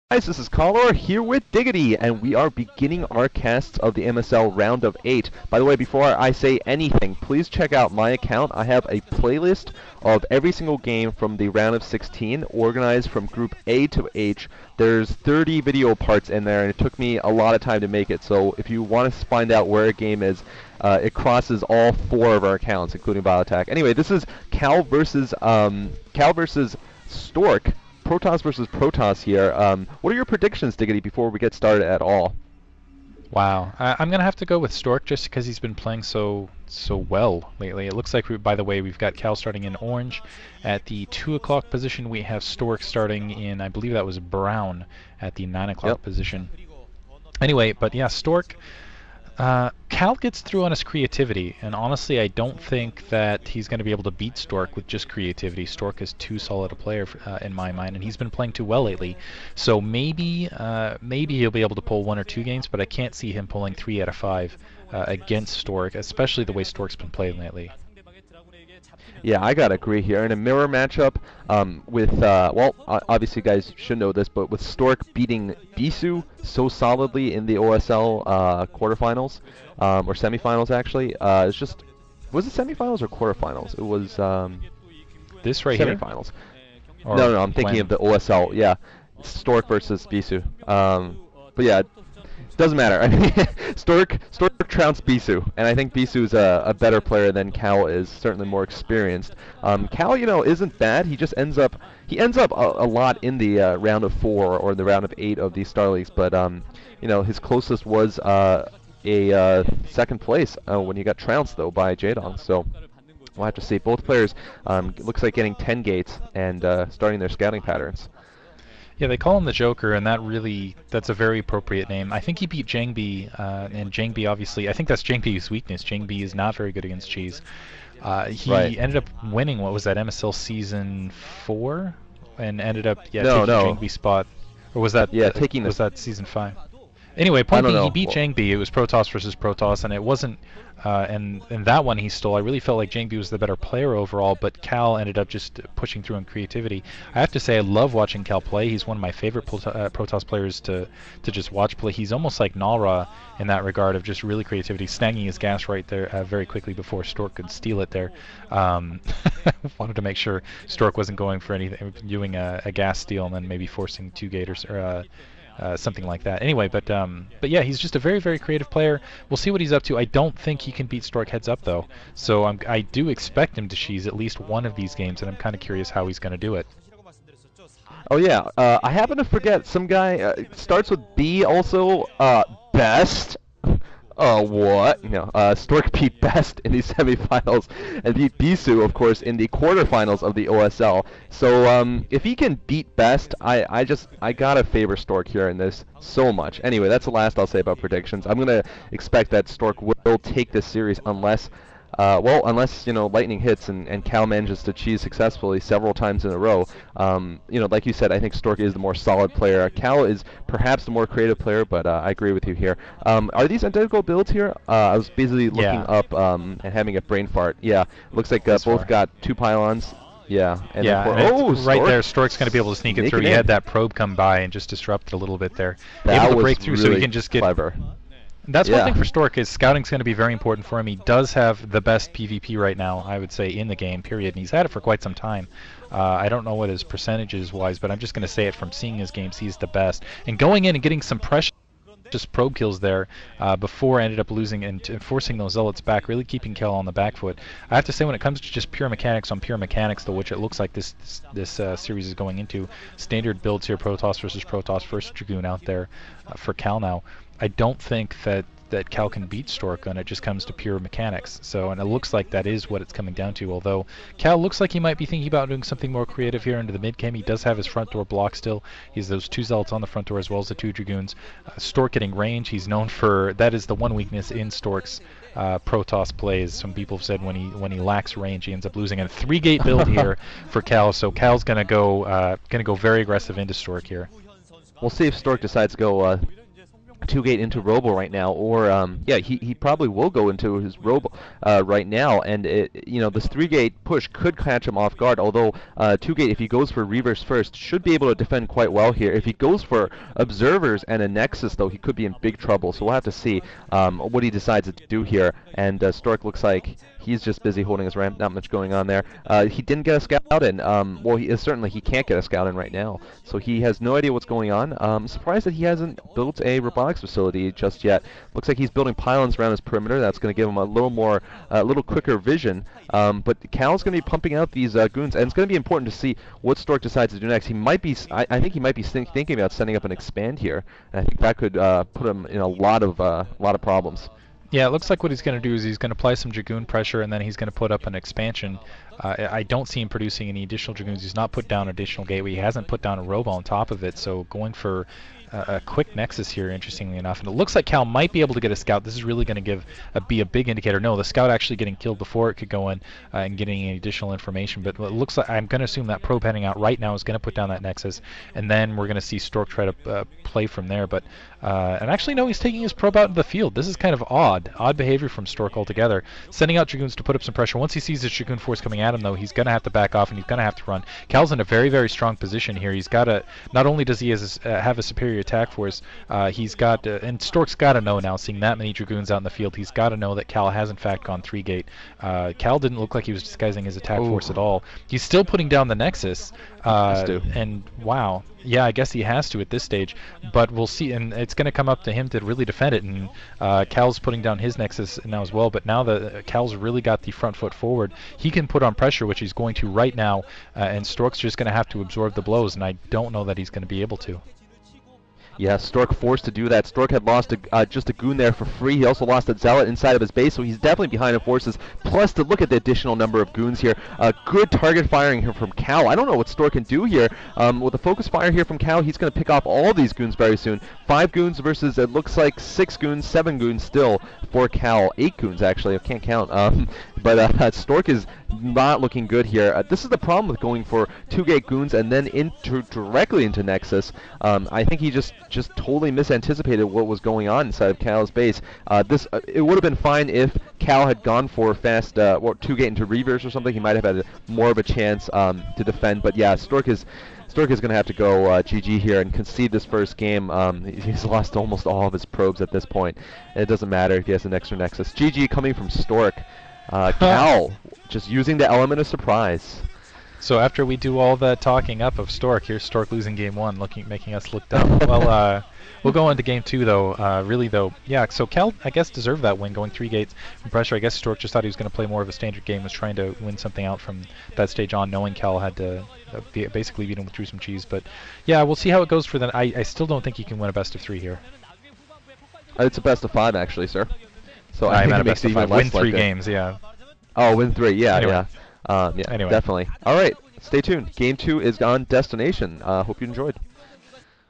ClubDay MSL Ro8 Stork v Kal Set 1
Set 1.  Map: Athena 2.  Kal: Orange P, 3. Stork: Brown P, 9.  Played 10/30/2008. Casted by Diggity and Cholera.